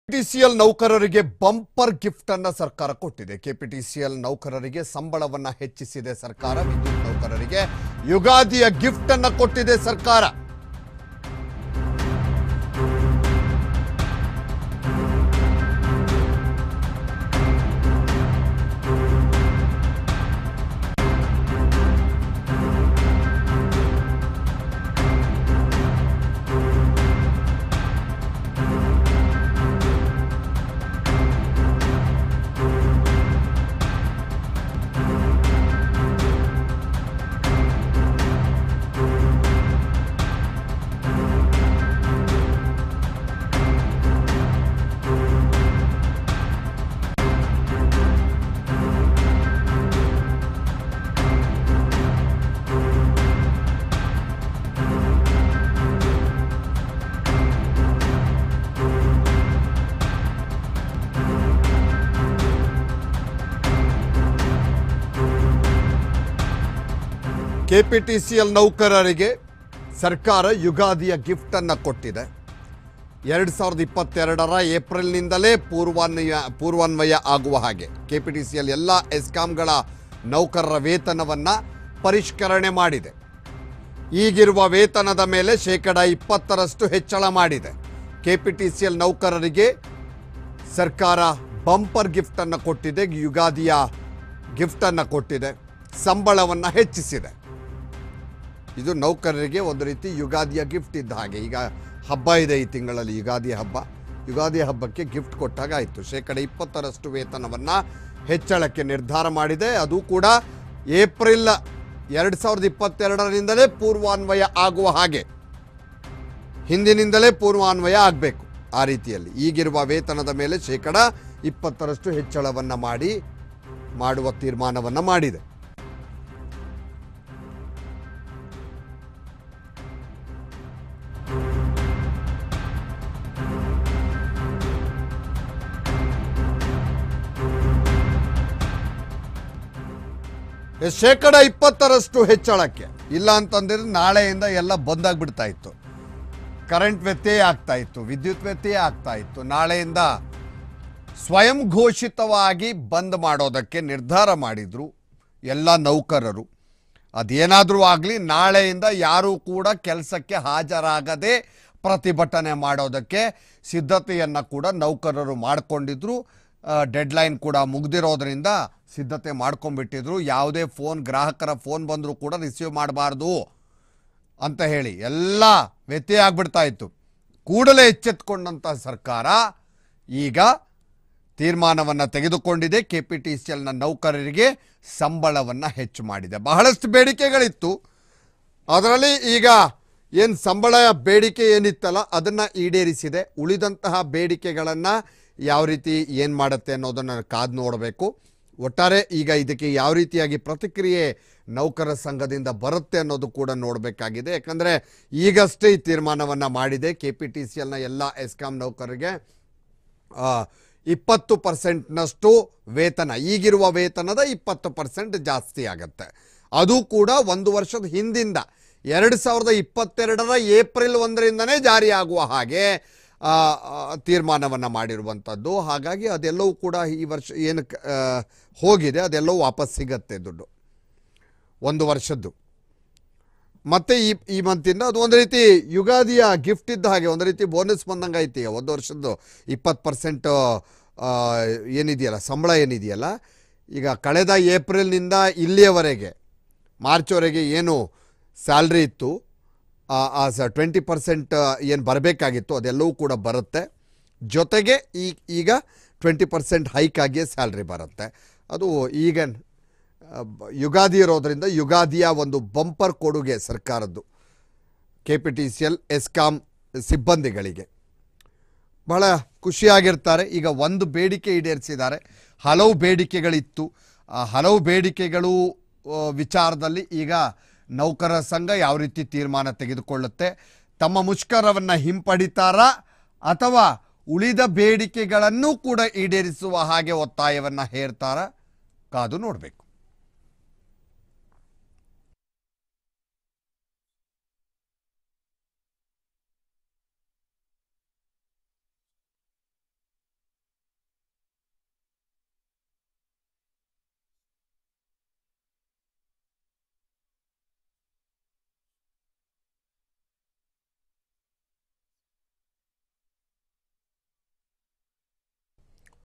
KPTCL ನೌಕರರಿಗೆ ಬಂಪರ್​ ಗಿಫ್ಟ್​​. KPTCL ನೌಕರರಿಗೆ ಸಂಬಳ ಹೆಚ್ಚಿಸಿದ ಸರ್ಕಾರ!
Btv News Kannada:KPTCL ನೌಕರರಿಗೆ ಬಂಪರ್​ ಗಿಫ್ಟ್​​. KPTCL ನೌಕರರಿಗೆ ಸಂಬಳ ಹೆಚ್ಚಿಸಿದ ಸರ್ಕಾರ. ವಿದ್ಯುತ್​ ನೌಕರರಿಗೆ ಯುಗಾದಿ ಗಿಫ್ಟ್ ಕೊಟ್ಟ ಸರ್ಕಾರ!